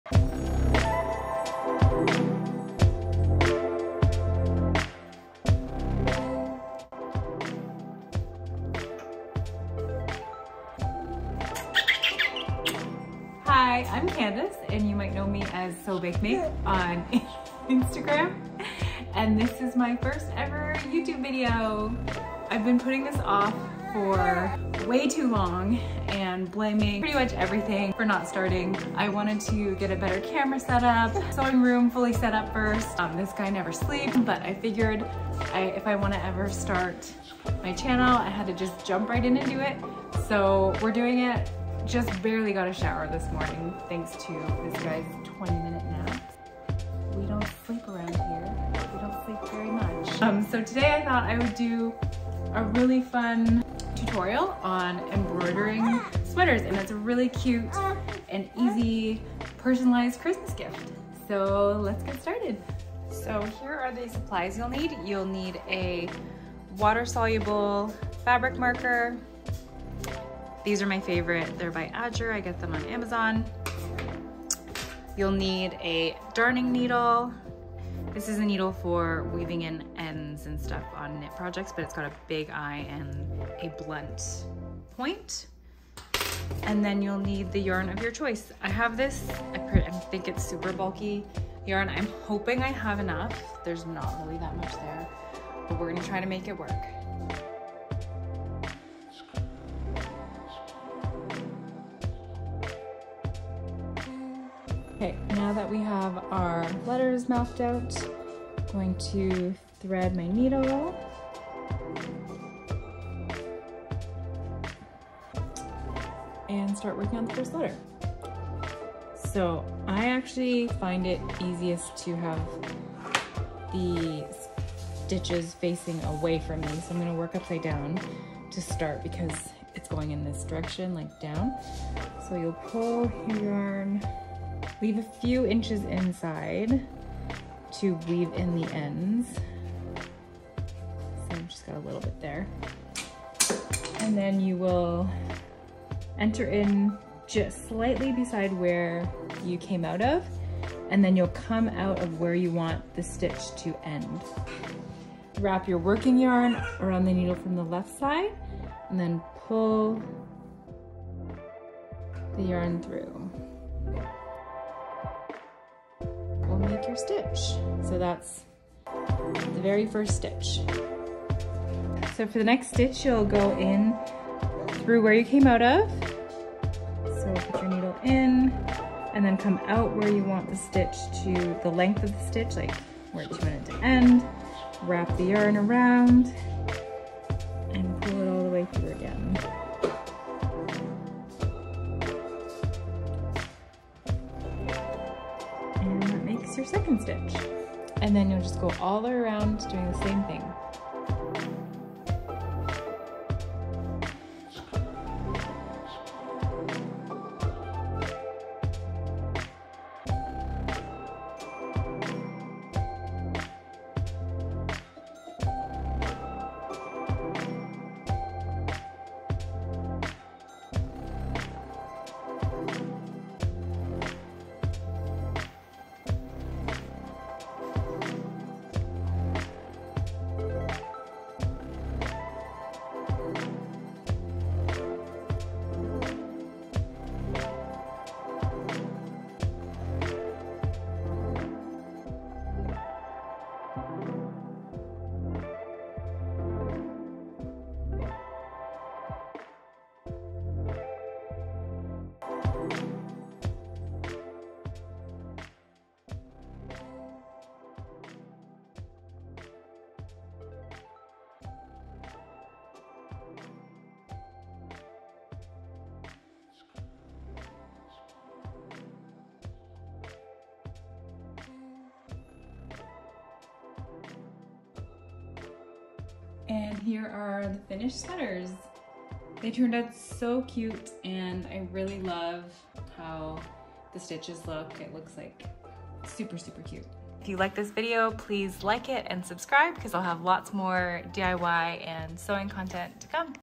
DIY How to Embroider with Yarn - Chain Stitch
In this video, I'm embroidering a personalized hand lettered style name onto a thick knit sweater with yarn. Hope you find this video helpful, and if you try out this technique, make sure to tag me on instagram so I can see!

Products Used:

Darning Needle
Water Soluble Fabric Marker
Yarn - Lion Brand Wool-Ease Thick & Quick

Timecodes:
0:00 - Intro
1:02 - Supplies you'll need
1:36 - Mark your Design out
1:44 - How to Chain Stitch
2:52 - How to Start the Next Stitch
4:16 - How to weave in ends
4:45 - Finished Sweaters